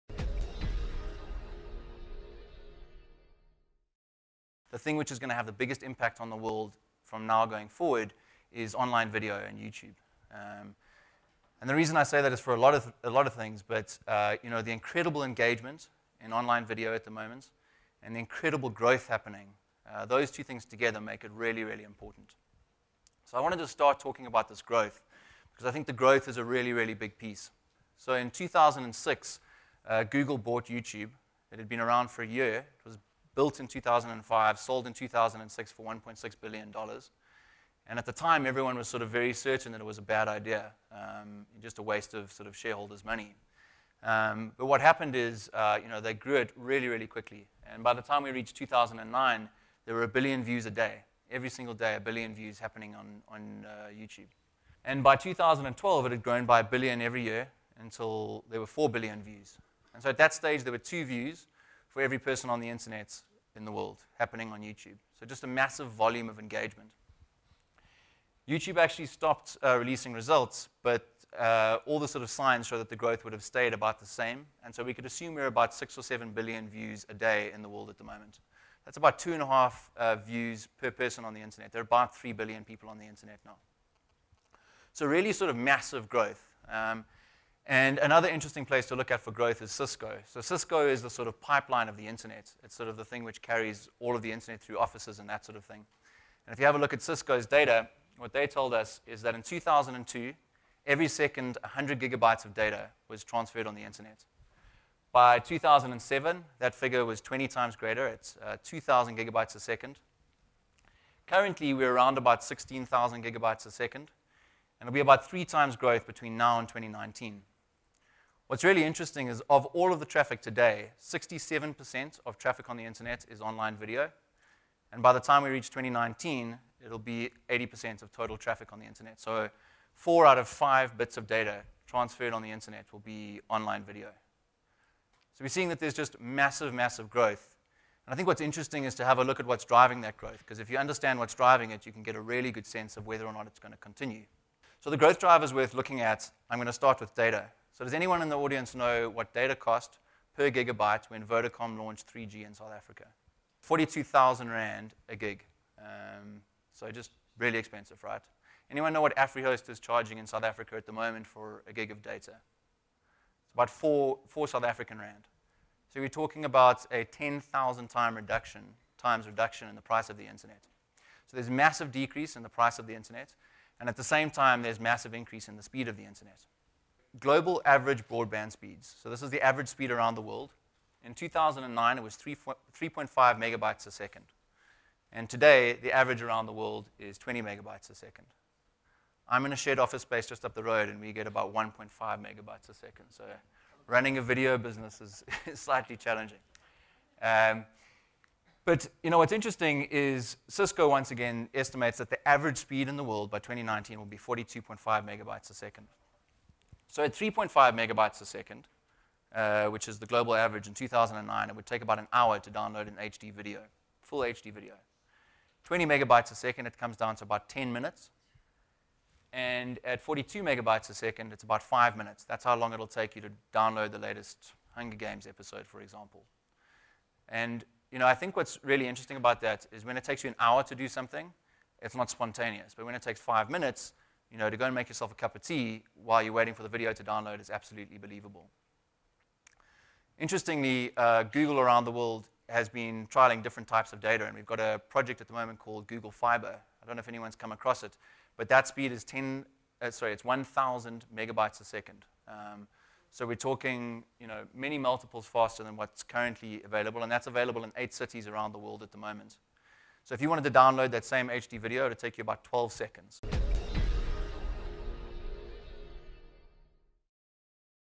Jon Ratcliffe - Online Video Part 1
According to Jon Ratcliffe, CEO of Mofilm Africa and previously Head of Agencies for Google and YouTube in Africa, online video is convincingly the fastest growing advertising segment in the world and will surpass TV (Part 1).